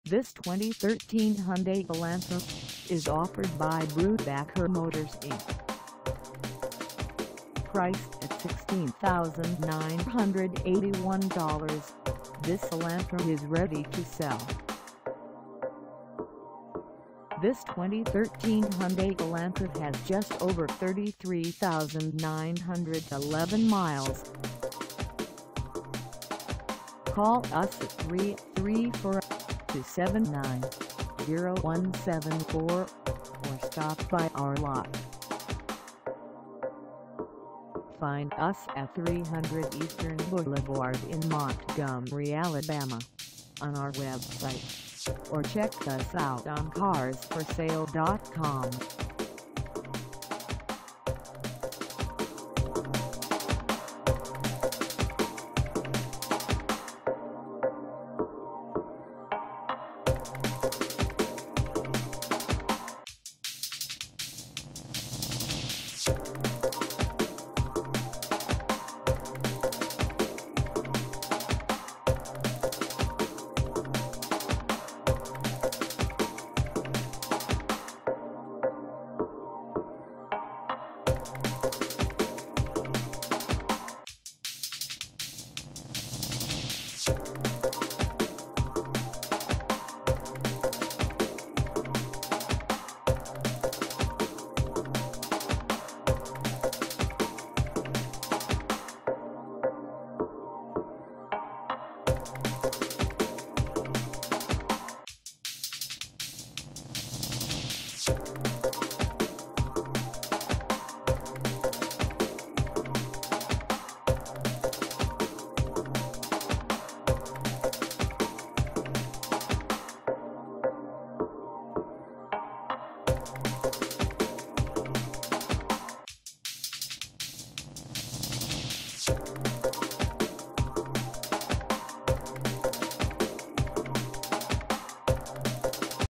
2013 Hyundai Elantra for sale in Montgomery, AL 36117 at BR
This 2013 Hyundai Elantra  is for sale in Montgomery, AL 36117 at BREWBAKER MOTORS INC.
Engine: 4-Cylinder I-4 cyl
Transmission: Unspecified
Color: Radiant Silver
Mileage: 33911
Contact BREWBAKER MOTORS INC 300 Eastern Blvd Montgomery, AL 36117 to test drive this 2013 Hyundai Elantra  today.